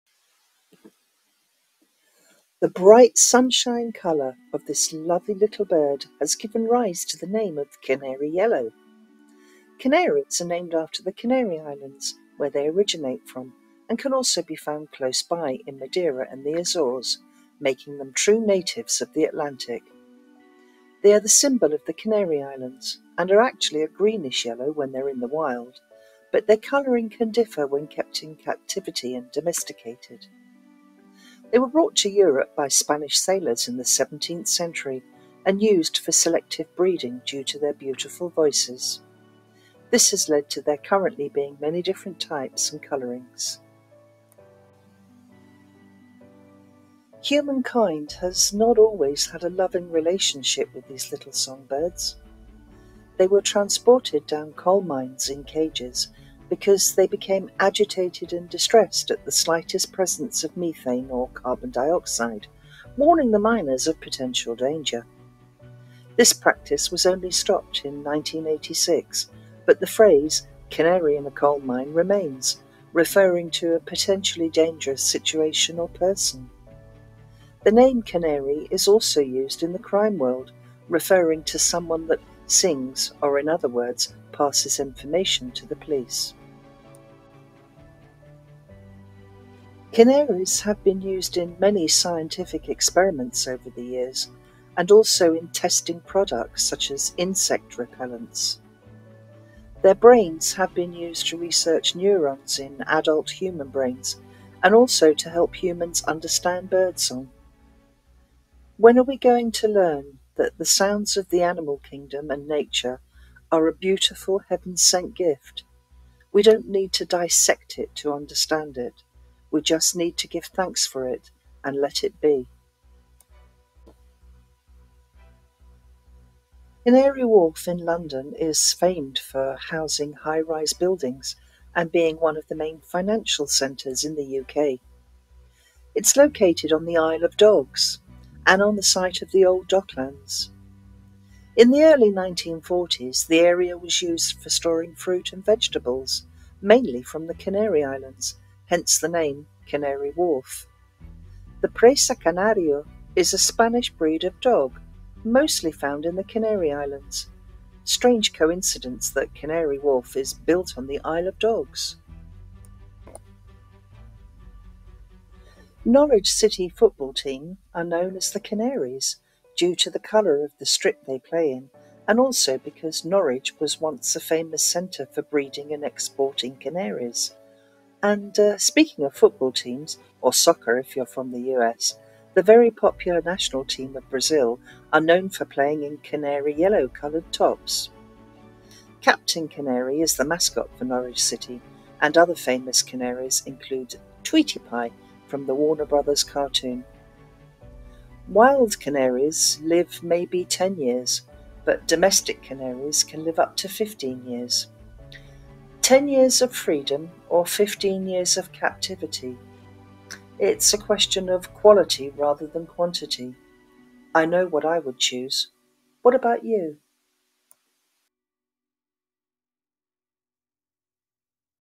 Canary - Shine On Little Songbird
Canaries are beautiful bright songbirds that have a long and chequered history with humankind. In the past we haven’t always treated them kindly. Let’s take a look at this one-sided relationship.